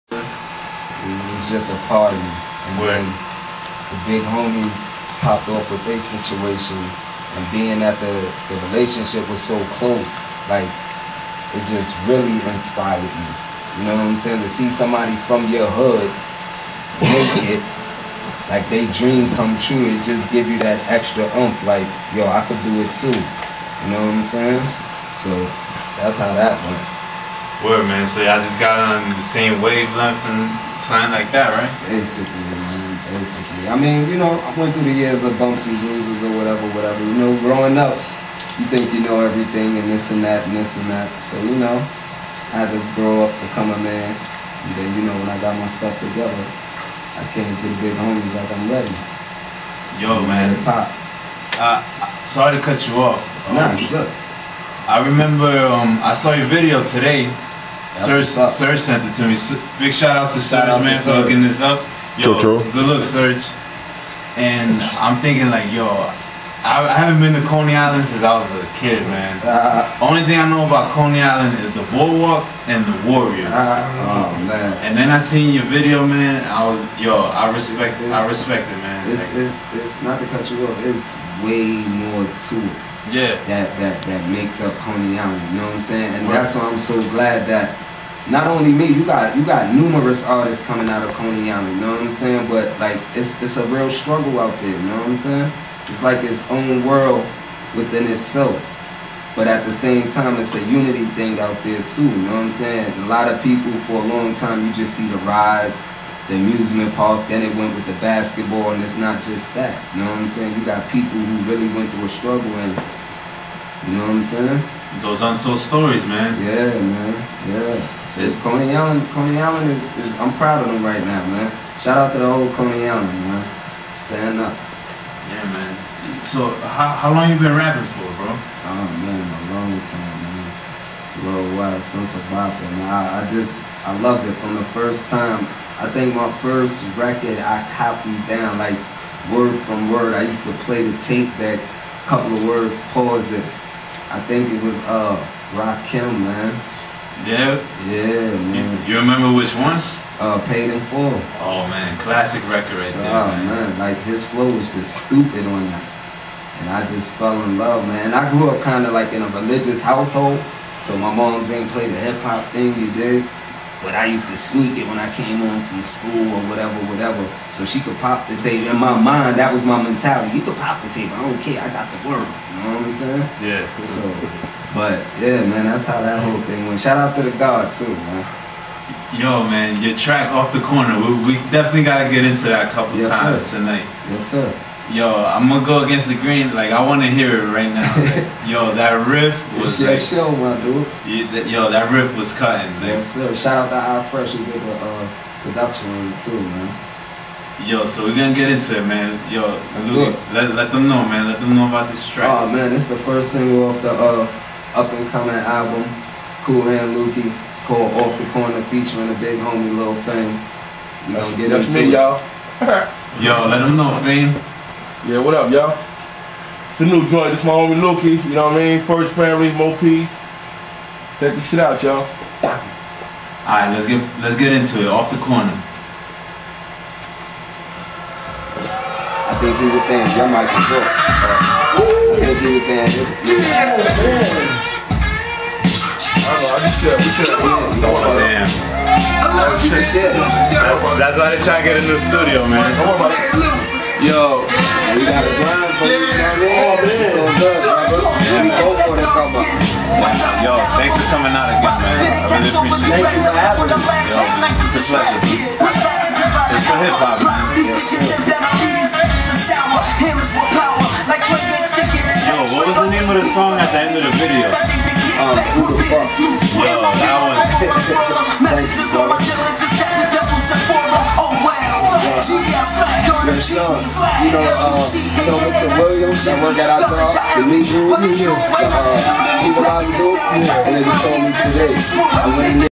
cool hand lukey interview with dj d at montclair state university radio
from nomeasuremusictv